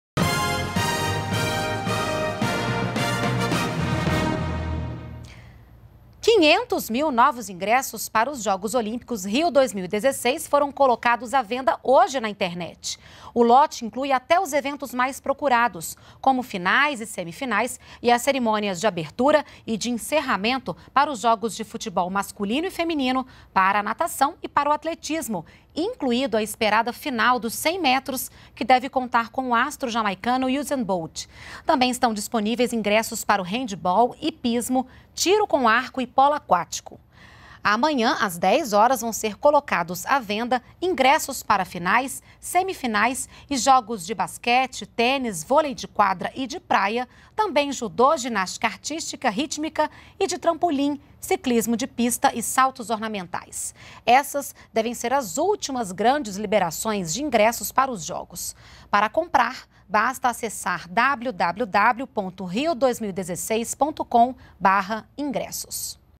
Novos ingressos para os jogos olímpicos são disponibilizados na internet
NBR NOTÍCIAS - 21.01.16: Nesta quinta-feira, foram disponibilizados na internet 500 mil novos ingressos para os jogos Rio 2016. A partir da próxima sexta-feira (22), serão colocadas à venda entradas para finais e semifinais, além de diversas modalidades. Serão as últimas grandes liberações de ingressos para os jogos.